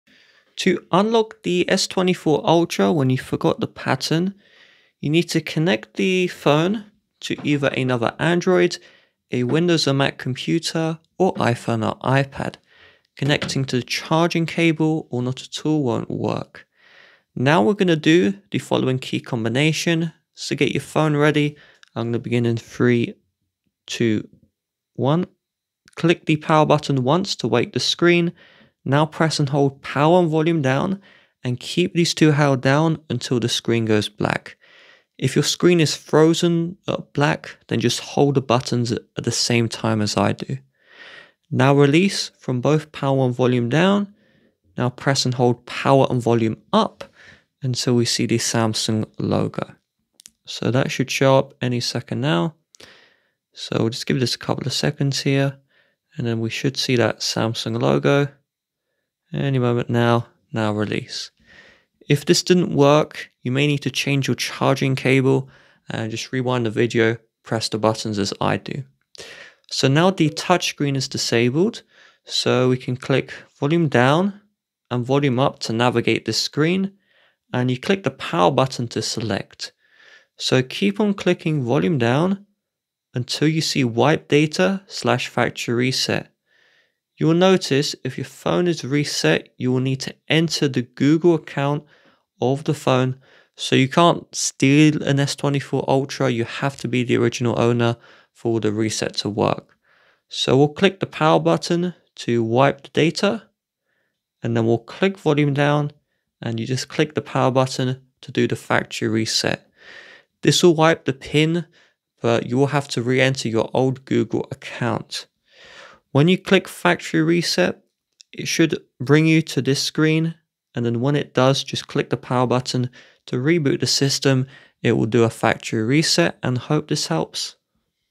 How To Unlock Forgot Pattern Samsung Galaxy S24 Ultra
Welcome to our guide on how to unlock your Samsung Galaxy S24 Ultra if you've forgotten your pattern! In this video, we’ll walk you through the steps to regain access to your device using alternative methods. We’ll cover options such as using your Google account, Samsung’s Find My Mobile service, and performing a factory reset if necessary. Follow along to unlock your Galaxy S24 Ultra and access your data securely even if you’ve forgotten your pattern.